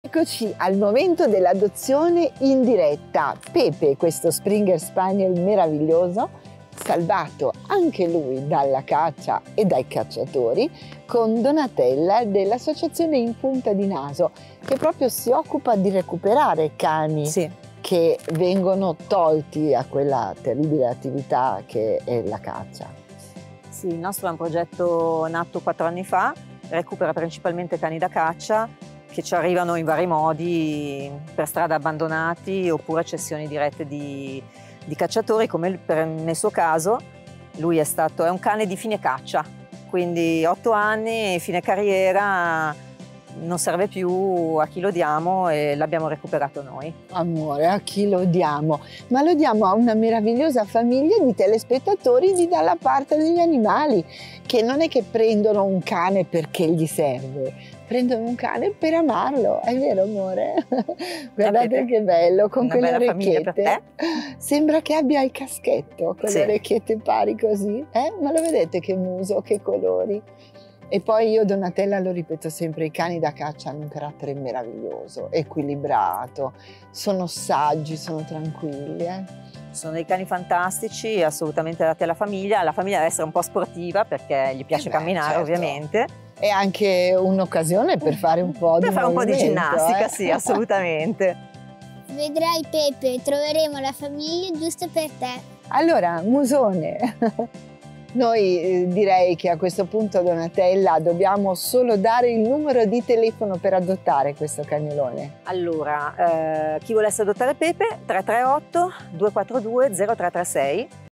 PUNTATA DEL 14 MAGGIO, ADOZIONE 2: PEPE
PEPE è un bellissimo springer spaniel: a otto anni è stato abbandonato da persone che lo sfruttavano solo per la caccia. Se volete dargli tanto amore non aspettate oltre! Chiamate subito In Punta di naso (MI) 📲 338/2420336